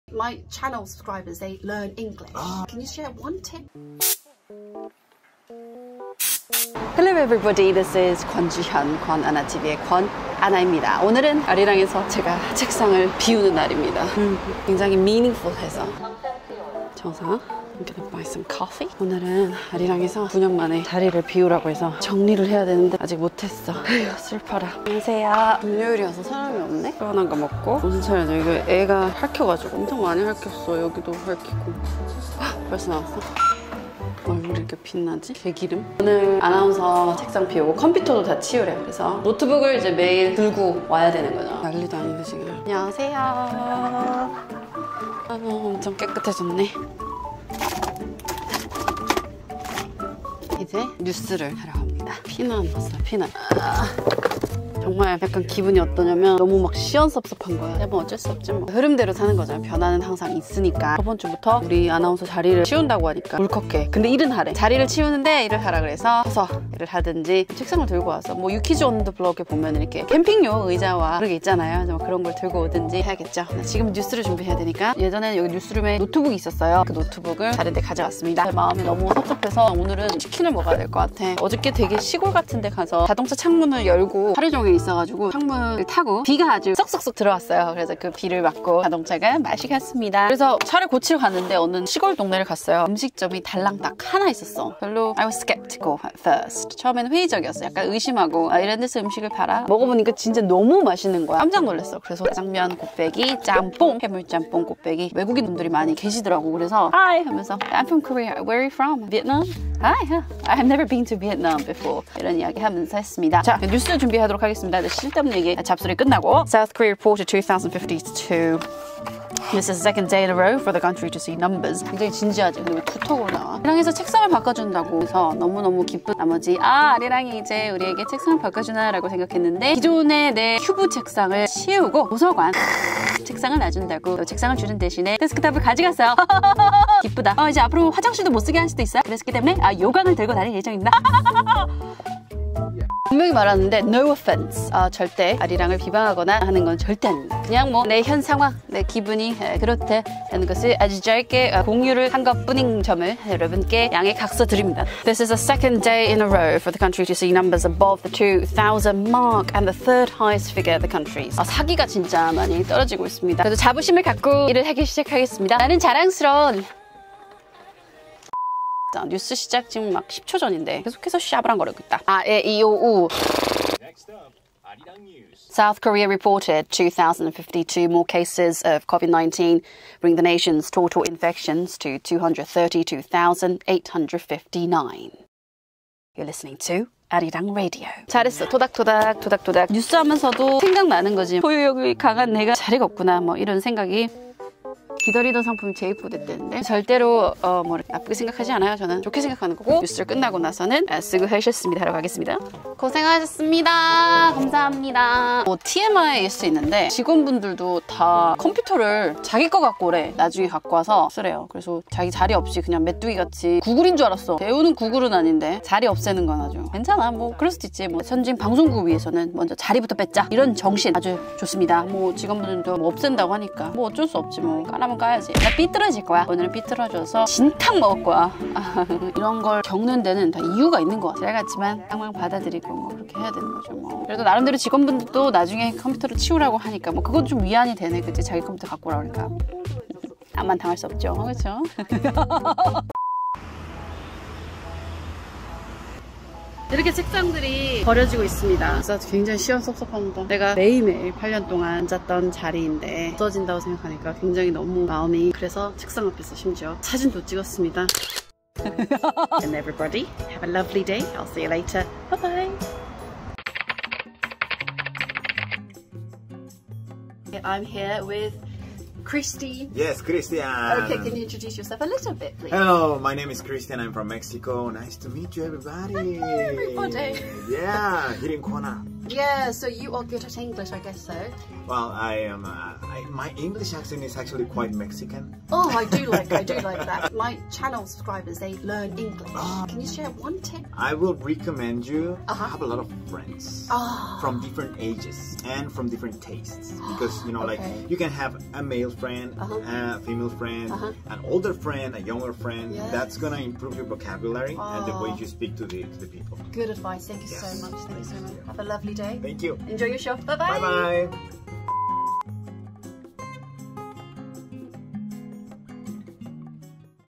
[영어회화 꿀팁을 알아보자] 비정상회담 | 크리스티안
오늘은 8년간 근무했던 아리랑에서 제 책상이 치워지던 날입니다.

그리고 비정상회담에 출연했던 크리스티안도 잠깐 만나 영어회화 방법에 대해 물어보았습니다.

영상 봐주셔서 항상 감사드립니다!!